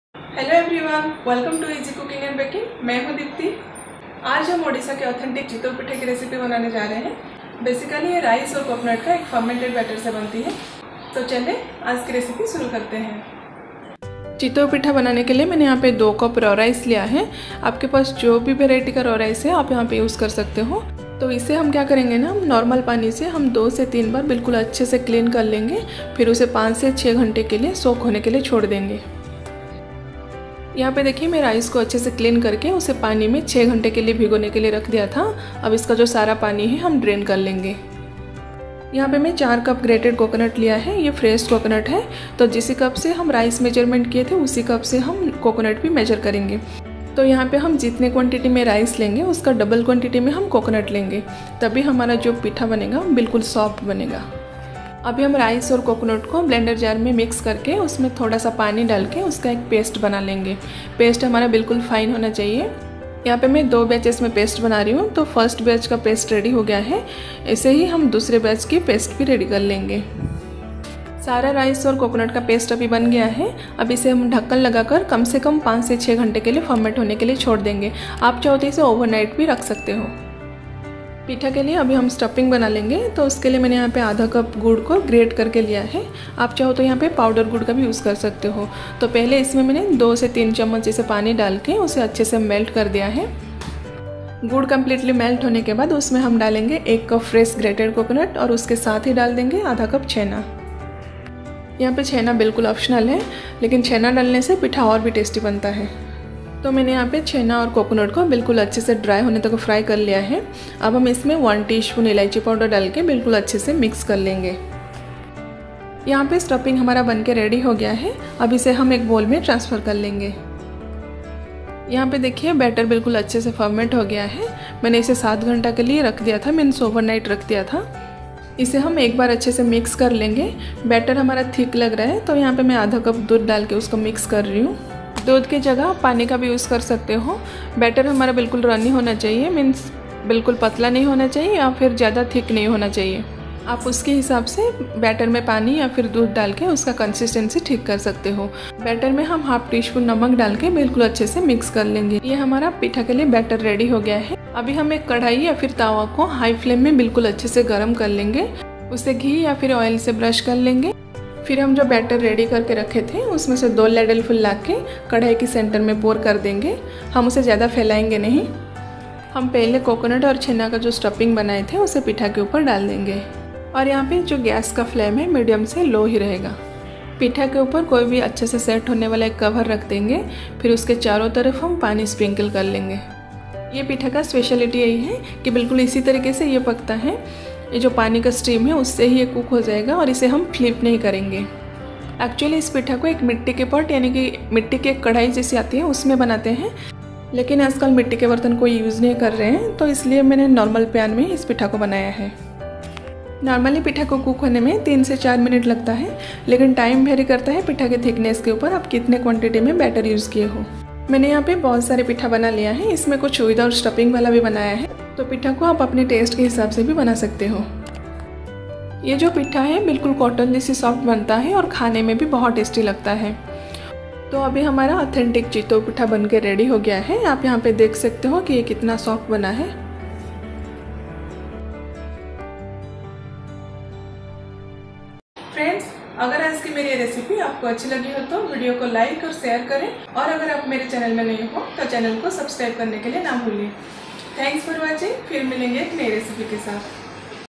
Odisha special Chitou pitha recipe#ଚିତଉପିଠା perfect Chitou pitha recipe#ଚିତାଲାଗି ଅମାବାସ୍ୟା special
Chitou pitha is a authentic recipe of Odisha. It's mostly prepared on 'CHITALAGI Amabasya or Chitou amabasya.

Ingredients :
raw rice(you can take basmati rice) -2cup
fresh grated coconut -4cups
salt to taste
ghee

For stuffing :
grated coconut -1cup
chhena-1/4cup(optional)
jaggery -1/2cup